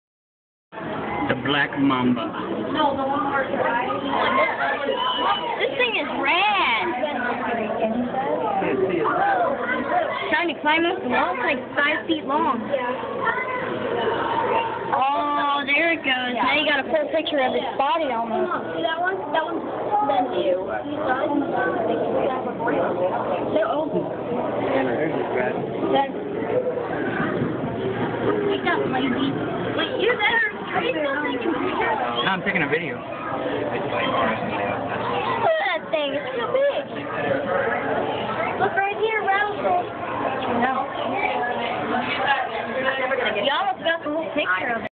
Black mamba at the st Louis zoo.
one of the deadliest snakes in the world, is scary up close...